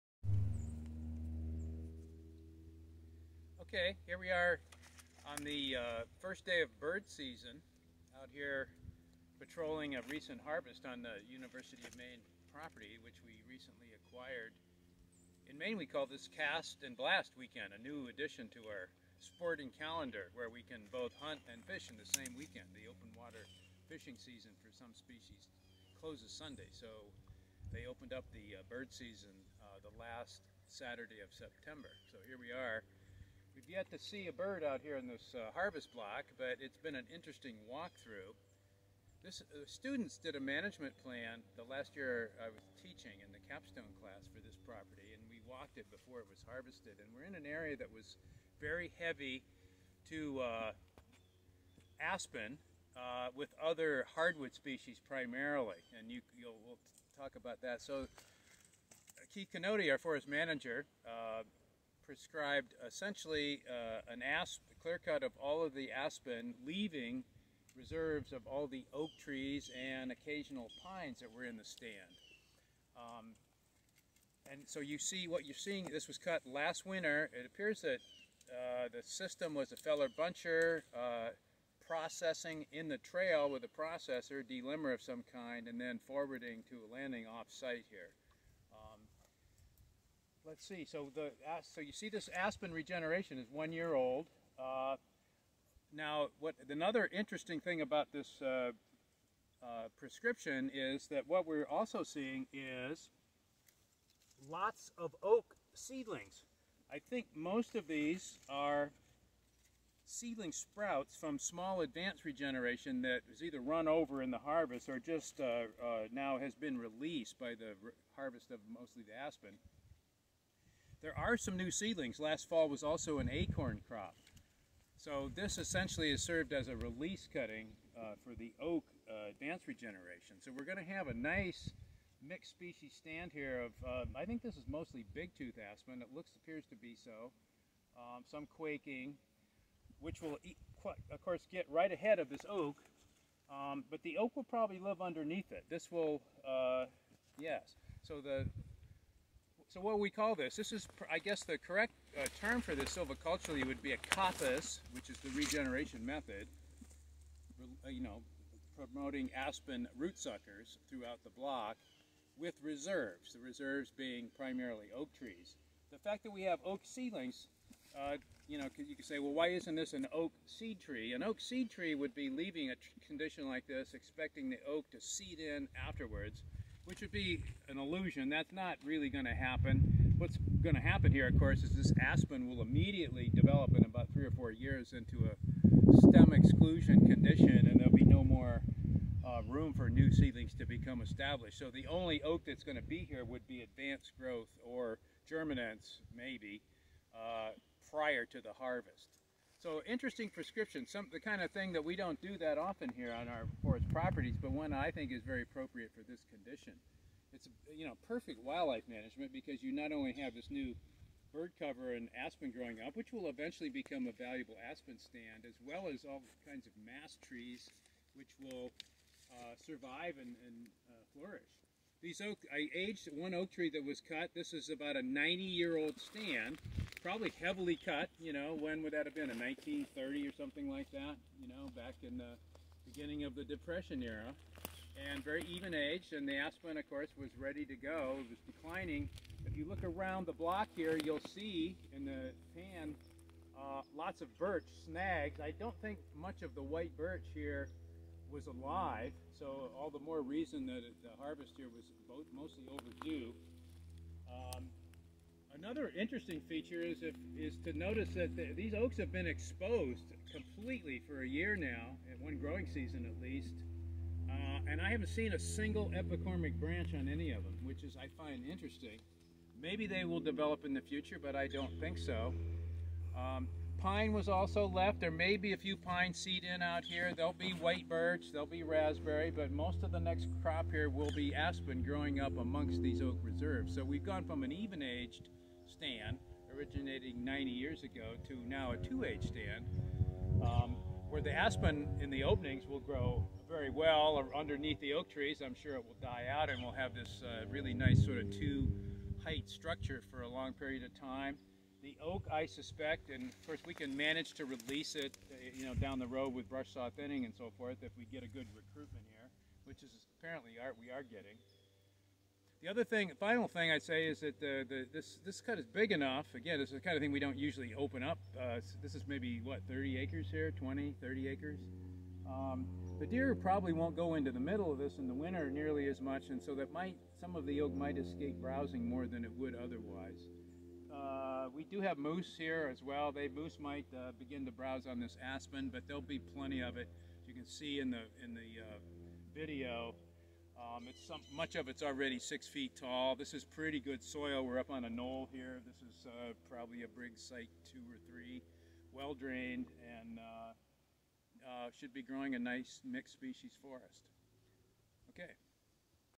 Aspen coppice clearcut with red oak reserves - UMaine's Lovejoy Lot
This shows the results after one growing season of an 11-acre (not 20-30 as I say in the video) coppice clearcut with reserves harvested last winter on the newly acquired Lovejoy Lot, a 50-acre parcel adjoining the much larger Henderson lot along Pushaw Stream  My Minnesota friends will feel right at home here!  The rest of the parcel not dominated by aspen was treated with an improvement cutting/shelterwood establishment treatment.

This is a regulatory clearcut in Maine because it is over 5 acres, has less than 30 ft2 of merchantable residual basal area, and does not have a well distributed sapling regeneration layer.  Category I (up to 20 acres) requires only that the surrounding separation zone (250 feet, on your own property) be in a non-clearcut status. For category II (20-75 acres) you need a separation zone equal in size to the area cut that is stocked with at least 60 ft2 of BA, at least 250 feet wide on all sides.  All clearcuts must meet a regeneration requirement by 5 years but this is almost never violated because all commercial species count.